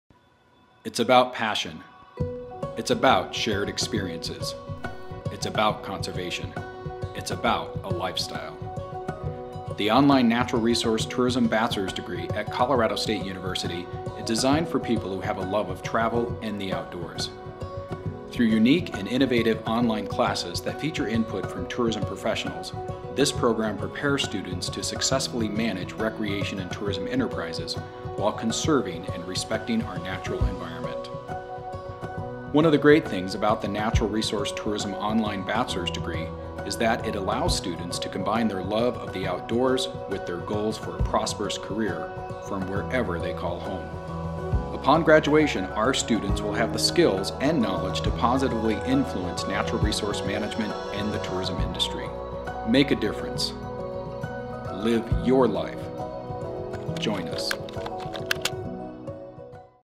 B.S. Natural Resource Tourism Online (Short) – Colorado State University
This online degree prepares students for careers in natural resource related tourism positions in the private, public, and non-profit sectors.

The curriculum emphasizes courses in tourism management, marketing and planning, natural resources, tourism business, and social sciences to develop appropriate skills for managing recreation and tourism enterprises. Which includes, but is not limited to, managing resorts, tour companies, outfitting and guiding companies, major corporations, ecotourism companies, cruise lines, state travel and tourism offices, national and international tourism offices, travel and tourism bureaus, hotel and lodging associations, meeting planner groups, and ski/tour associations.

The B.S. in Natural Resource Tourism is offered through CSU Online by The Department of Human Dimensions of Natural Resources in the Warner College of Natural Resources at Colorado State University.